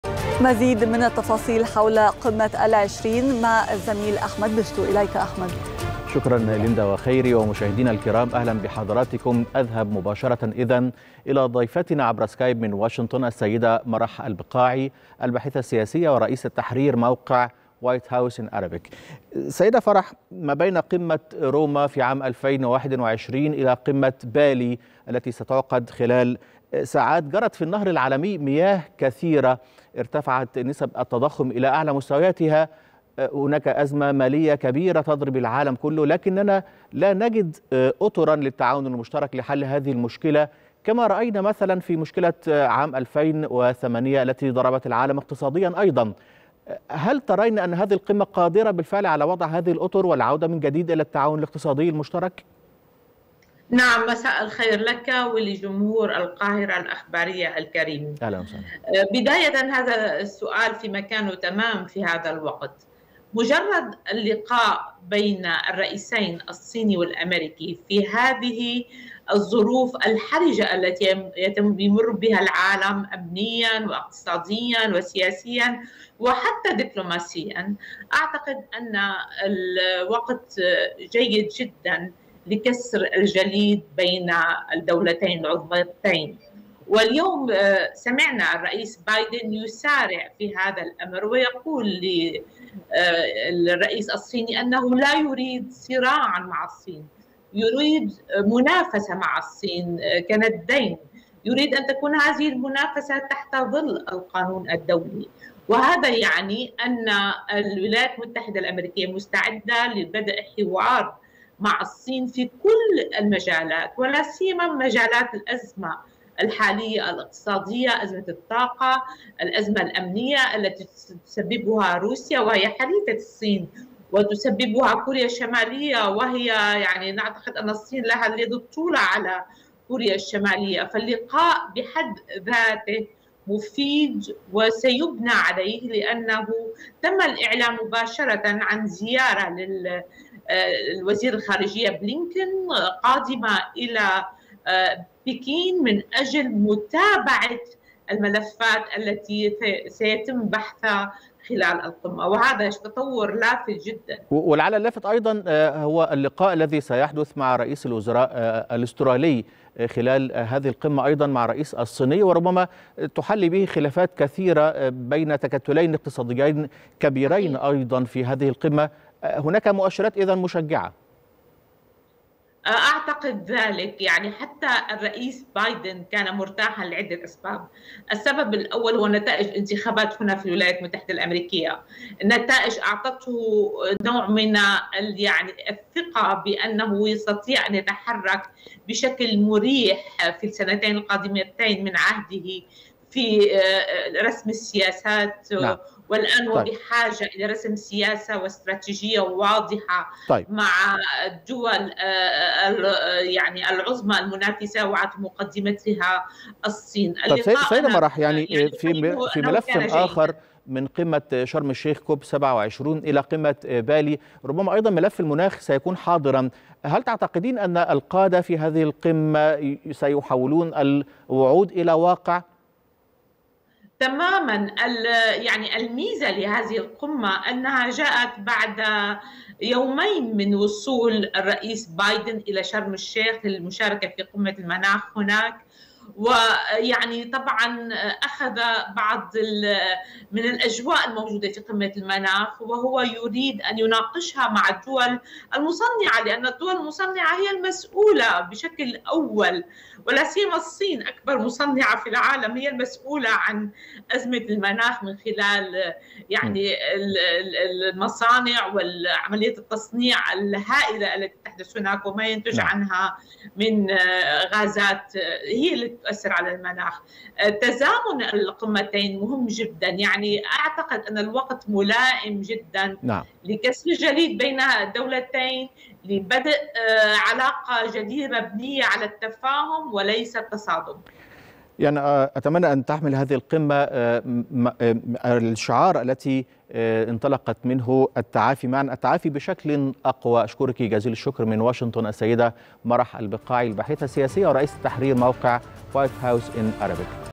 من واشنطن ..مرح البقاعي الباحثة السياسية: الوقت الحالي جيد لكسر الجليد بين أمريكا والصين
١٥-١١-٢٠٢٢

هنا_القاهرة_عاصمة_الخبر 15/11/2022 15/11/2022